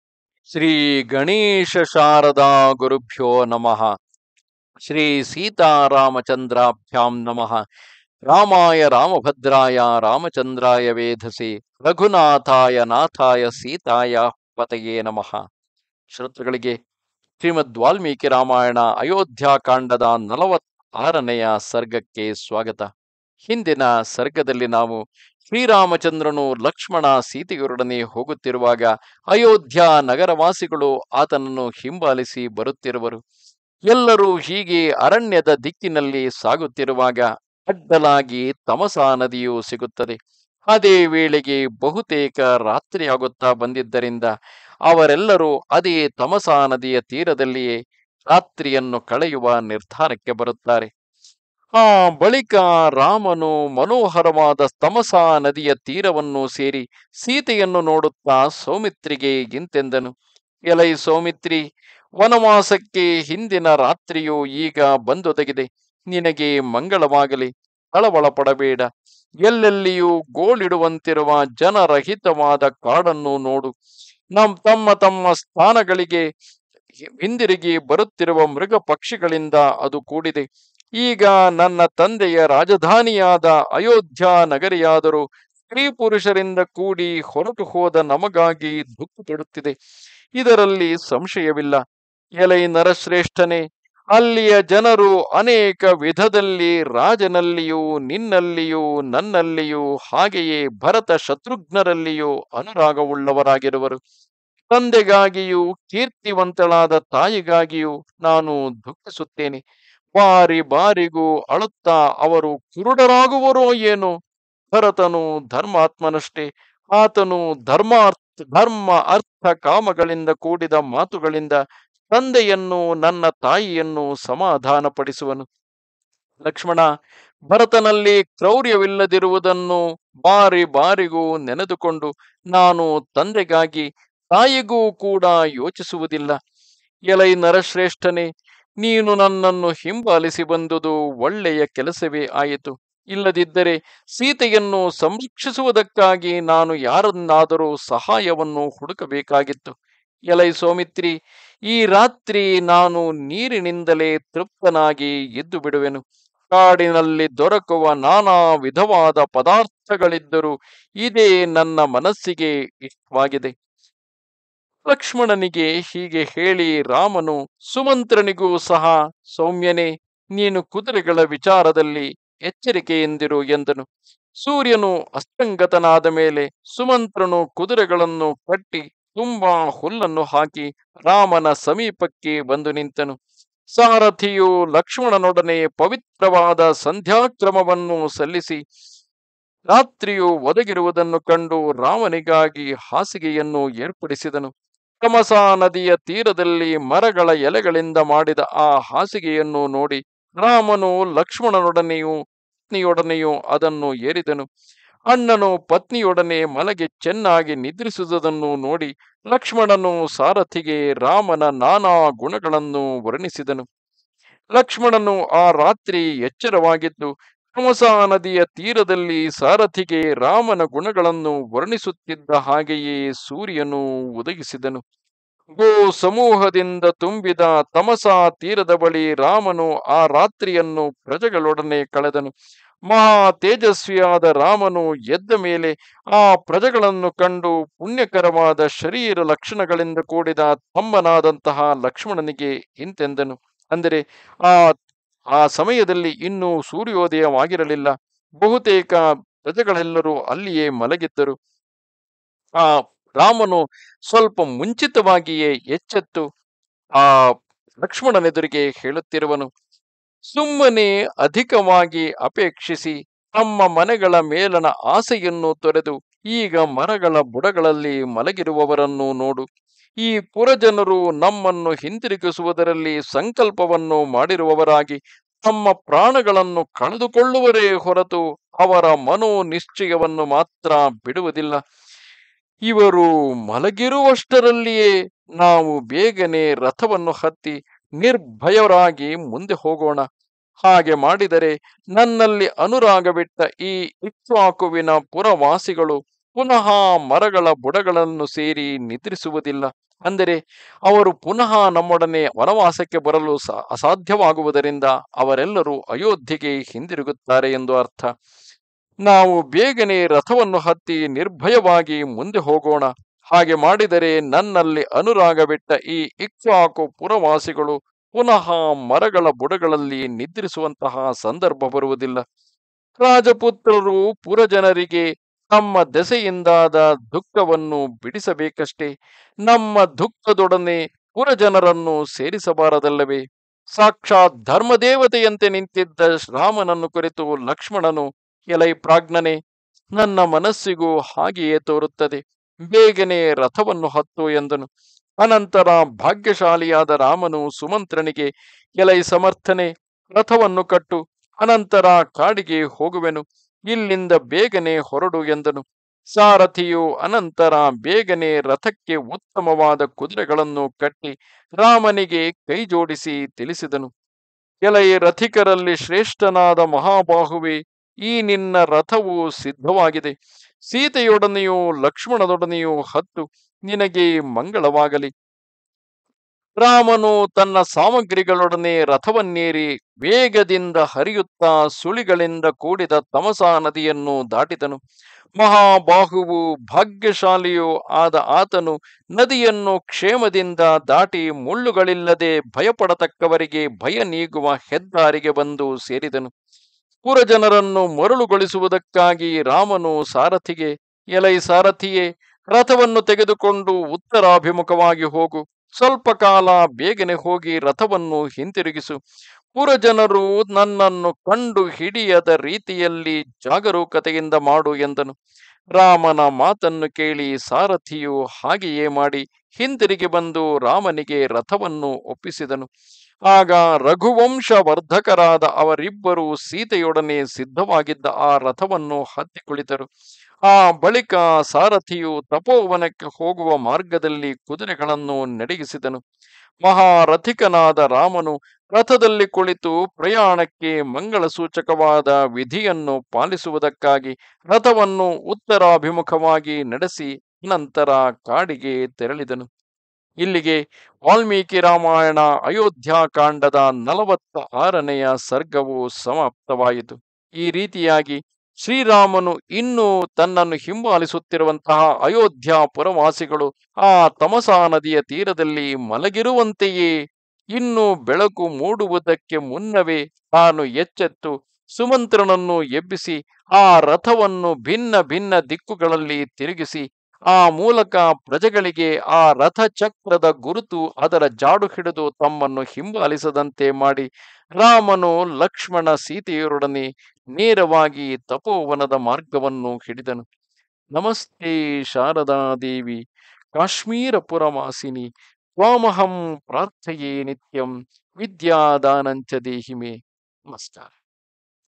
Ramayana | ರಾಮಾಯಣ | ಅಯೋಧ್ಯಾಕಾಂಡ | ಸರ್ಗ – ೪೬ | ಪೌರರನ್ನು ಬಿಟ್ಟು ರಾಮನು ಅರಣ್ಯಕ್ಕೆ ಪ್ರಯಾಣ ಮಾಡಿದುದು
ಸೀತಾ – ಲಕ್ಷ್ಮಣರೊಡನೆ ಶ್ರೀರಾಮನು ತಮಸಾ ನದಿಯ ತೀರದಲ್ಲಿ ಬೀಡು ಬಿಟ್ಟುದು.  ತಂದೆ – ತಾಯಿಯರ ಮತ್ತು ಅಯೋಧ್ಯೆಯ ವಿಷಯವಾಗಿ ಚಿಂತಿಸಿದುದು.  ಅಯೋಧ್ಯಾವಾಸಿಗಳು ಮಲಗಿರುವಾಗ ಅವರನ್ನು ಬಿಟ್ಟು ಕಾಡಿನ ಕಡೆಗೆ ಪ್ರಯಾಣ ಮಾಡಿದುದು.